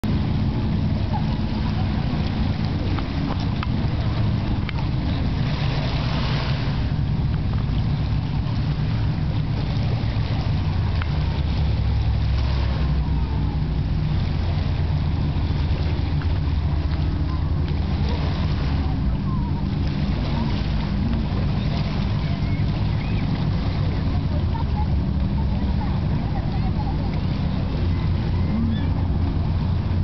Lake Pleasant Swimming 7/23/06
after Corina was bitten by fish, didn't want to go back to get her toy.